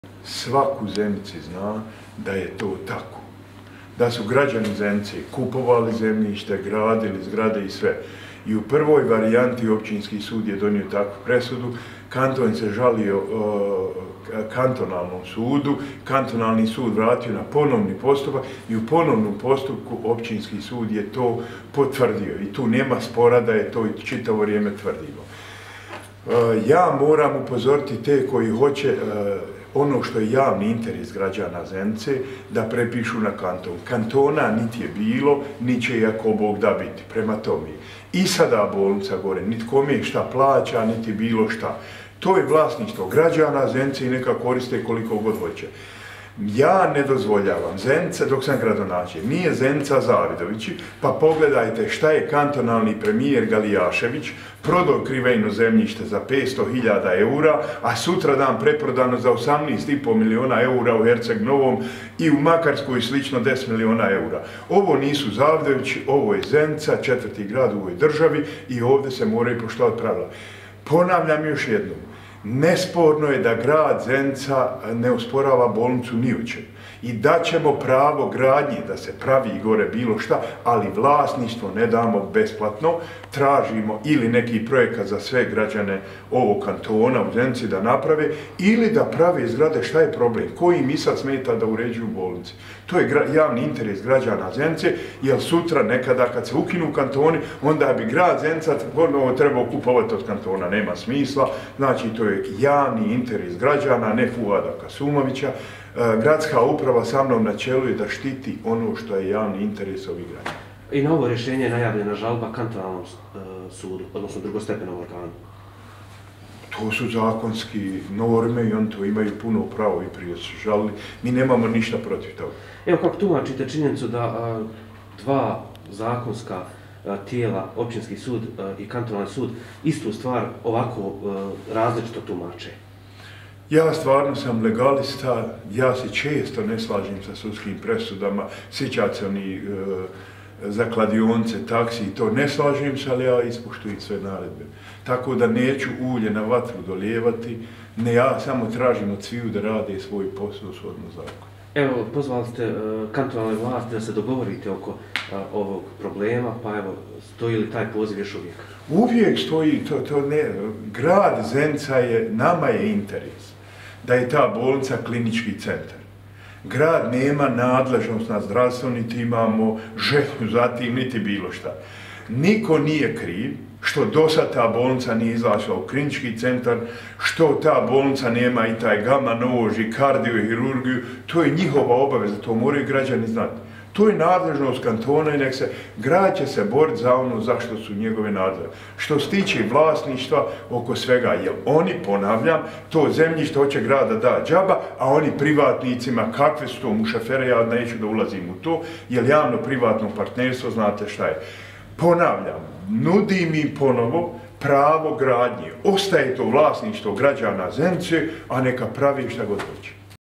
Zenica.ba - Fuad Kasumović - Izjava za RTV ZENICA - 06.05.2020.
Nakon što je Općinski sud u Zenici rješenjem od 9. marta ove godine potvrdio da su nekretnine koje trenutno koristi JU Kantonalna bolnica Zenica i na kojima planira realizaciju projekata povećanja energetske efikasnosti u JU Kantonalna bolnica Zenica te projekata javno-privatnog partnerstva, u vlasništvu Grada Zenica, javnosti se obratio i gradonačelnik Zenice Fuad Kasumović.

"Svima u Zenici je poznato da su nekretnine koje koristi Kantonalna bolnica Zenica vlasništvo Grada, jer su građani Zenice kupovali to zemljište i na njemu gradili objekte. Općinski sud u Zenici je već jednom donio rješenje da se dozvoljava upis ovih nekretnina u korist Grada Zenica. Međutim, JU Kantonalna bolnica Zenica izjavila je žalbu i Kantonalni sud Zenica donio je rješenje kojim se predmet vraća na ponovni postupak. Ali i u ponovljenom postupku Općinski sud Zenica potvrdio je da su predmetne nekretnine vlasništvo Grada Zenica", kazao je gradonačelnik Kasumović.

Naveo je kako se ovdje radi o javnom interesu građana Zenice te da ne može biti govora o tome da se nekretnine uknjižuju kao vlasništvo Zeničko-dobojskog kantona. Obrazložio je to na način da u vrijeme kada su građani Zenice svoja sredstva izdvajali za kupovinu zemljišta kantoni nisu ni postojali. Također je iznio svoj stav prema kojem smatra da je opstanak kantonalnog uređenja Federacije BiH neizvjestan.

Gradonačelnik Kasumović je naglasio kako JU Kantonalna bolnica Zenica i sada koristi nekretnine bez ikakve naknade.

Dalje je dodao: "Dok sam ja gradonačelnik Zenice, niko nesavjesno neće upravljati imovinom građana Zenice. Pravila se moraju poštovati. Gradska uprava Zenice spremna je podržati projekte Kantonalne bolnice Zenica. S naše strane neće biti nikakvih opstrukcija. Spremni smo dati bolnici pravo građenja, ali vlasništvo na nekretninama ne možemo dati besplatno. Zadatak Gradske uprave Zenica i mene kao gradonačelnika je da štitimo javni interes naših građana".

Ponovio je kako je osnovni uslov Grada Zenica za prenos vlasništva nad zemljištem i objektima u krugu JU Kantonalna bolnica Zenica da Vlada Zeničko-dobojskog kantona preuzme obavezu finansiranja realizacije jednog infrastrukturnog projekta od društvenog značaja za sve građane Zenice.

Osvrnuo se i na činjenicu da je JU Kantonalna bolnica Zenica i na posljednje rješenje Općinskog suda Zenica doneseno u korist Grada Zenica uložila žalbu.

"Kao legalista smatram da je legitimno pravo Kantonalne bolnice da iskoristi dozvoljene zakonske odnosno pravne lijekove. Gradska uprava Zenice ne komentariše i ne raspravlja o odlukama sudova. Nismo to dosad činili, pa nećemo ni ovaj put", zaključio je gradonačelnik Fuad Kasumović.

Na kraju je istakao da je Gradska uprava Zenica još uvijek otvorena da se pitanje vlasničko-pravnih odnosa na nekretninama koje koristi JU Kantonalna bolnica Zenica riješi sporazumnim putem te da je interes Grada Zenica da bolnica u Zenici postane klinički centar i da svi građani Zenice, kao i cjelokupnog područja Zeničko-dobojskog kantona imaju vrhunske zdravstvene usluge.